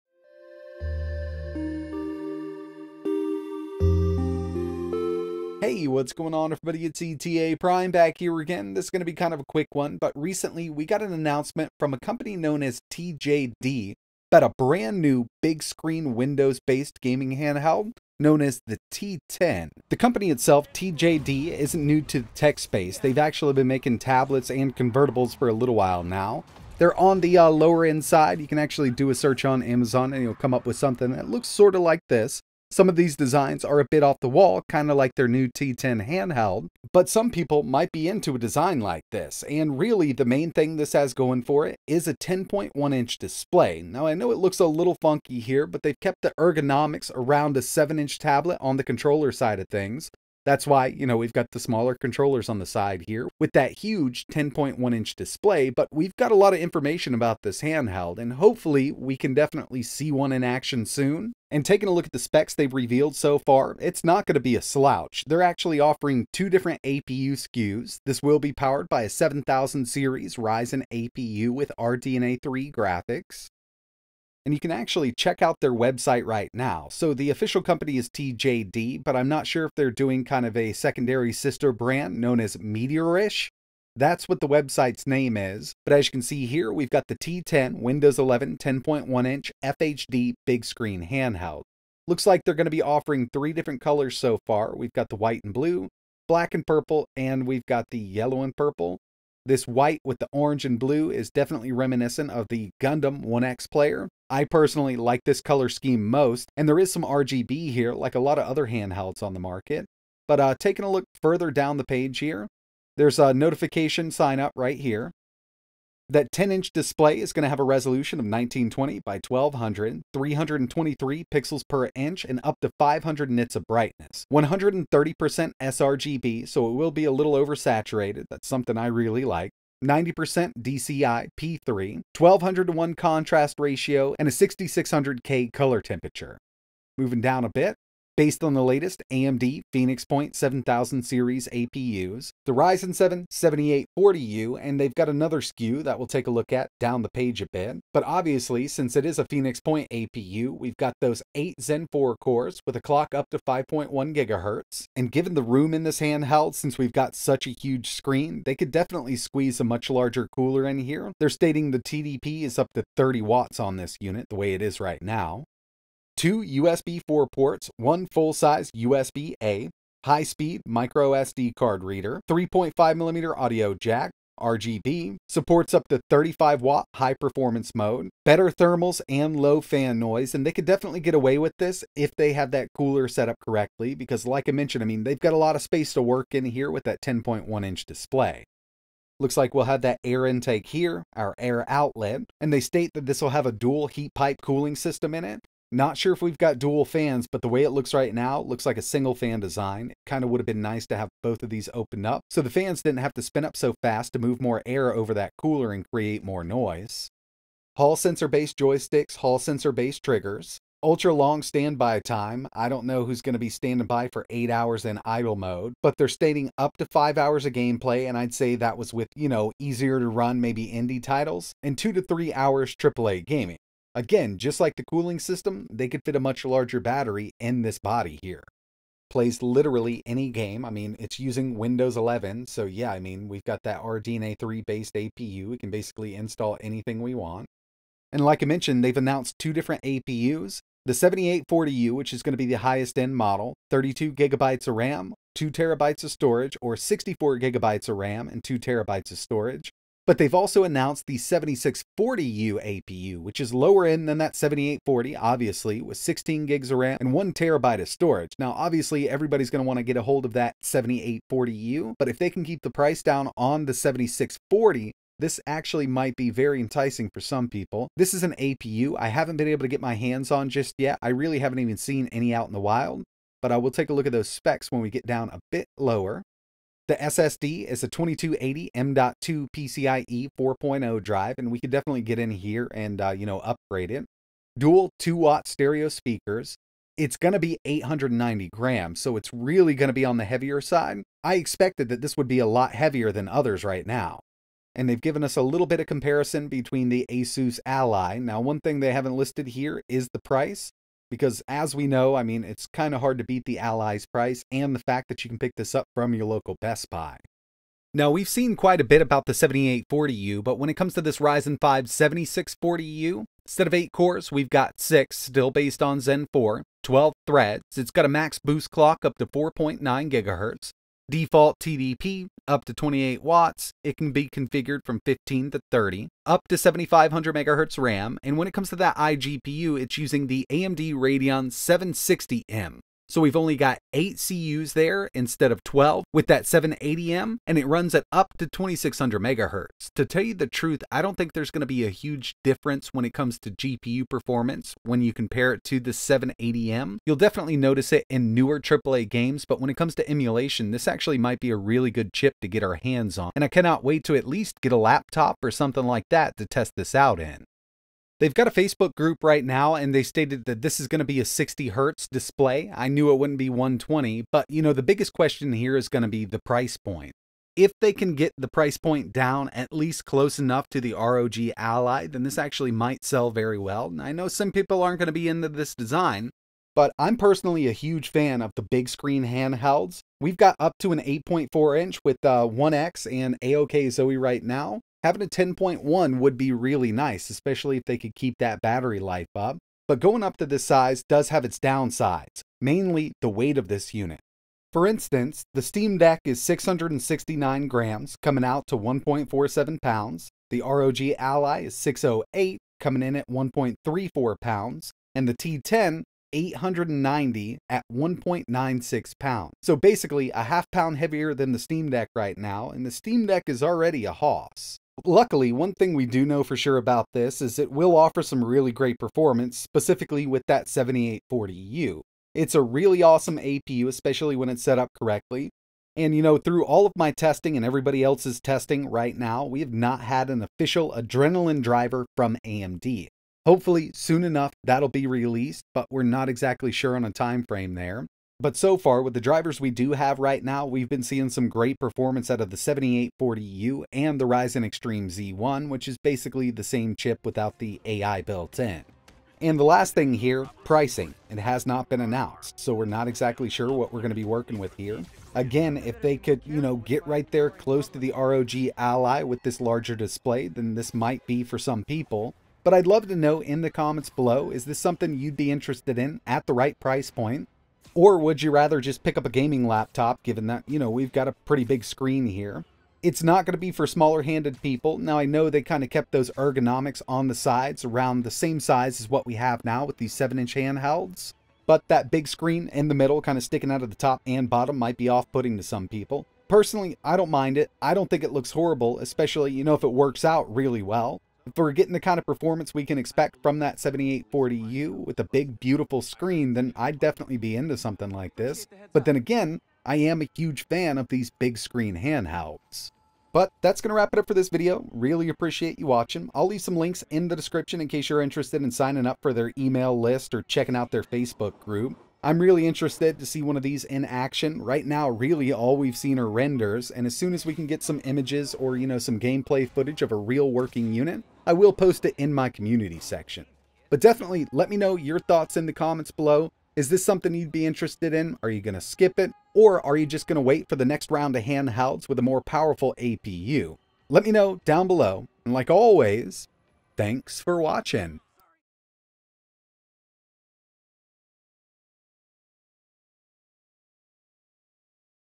An All-New Big Screen X86 Hand-Held Is Coming Soon! It’s Called The TJD T10
An all new Big Screen handheld is on the way from a company called TJD known as the Meteorish T10. Powered by the Ryzen 7 7840U and an option for the Ryzen 5 7640U! It’s rocking a 10.1 inch ips display up to 64 GB of wam and a 60W battery! It’s looking like an interesting Ryzen Handheld gaming PC.

25% Code for software: ETA

ETAPRIME
12400 Wake Union Church Rd PMB #239